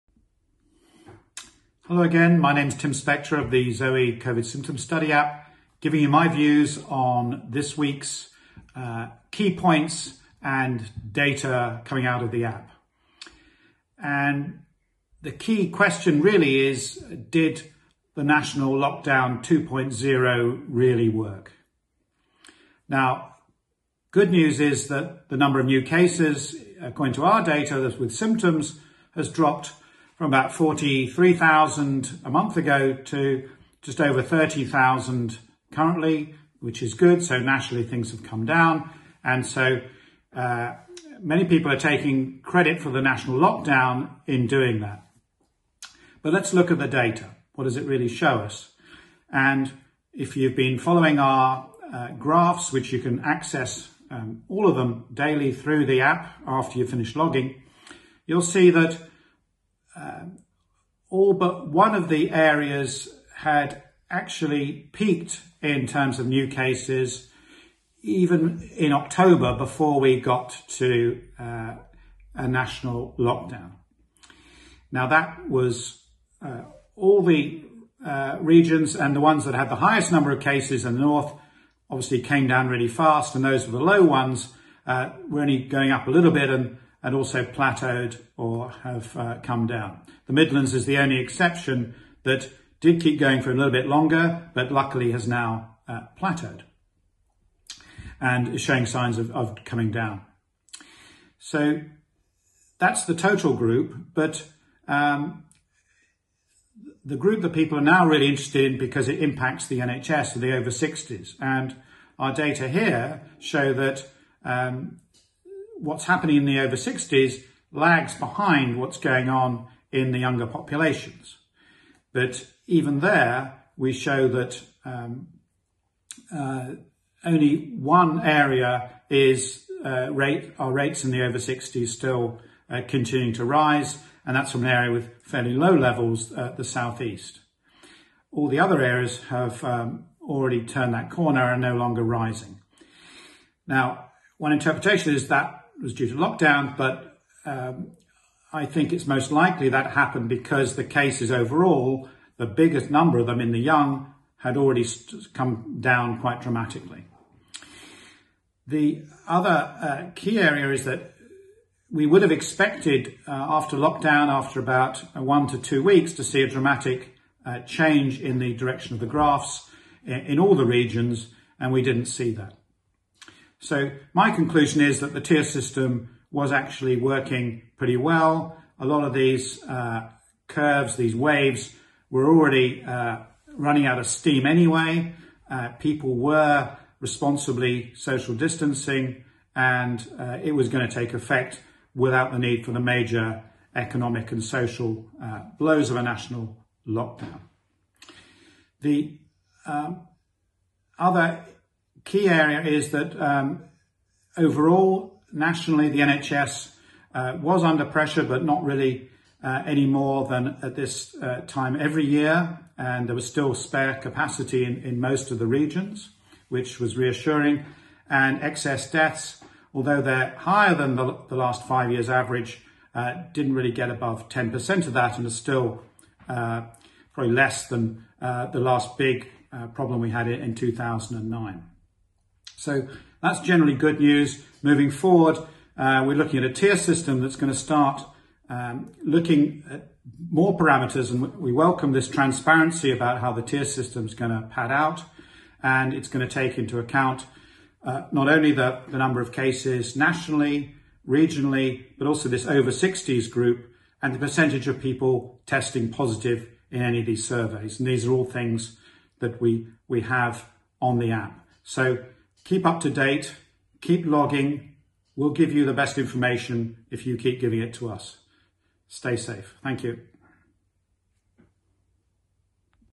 Did lockdown work?
Professor Tim Spector from the ZOE COVID Symptom Study app takes a look at the data to see whether lockdown worked.

If you need subtitles, please click the option for subtitles at the bottom right of the video.